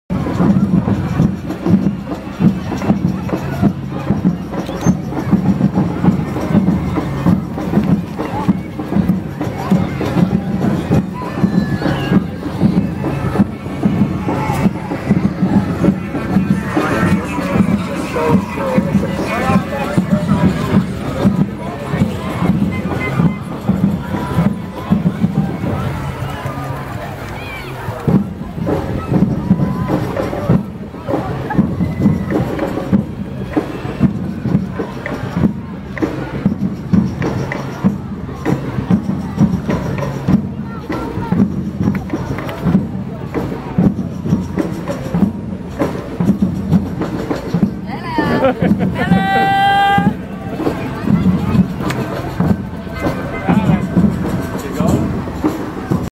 Broome Shinju Matsuri Parade 2015
crazy bike at the parade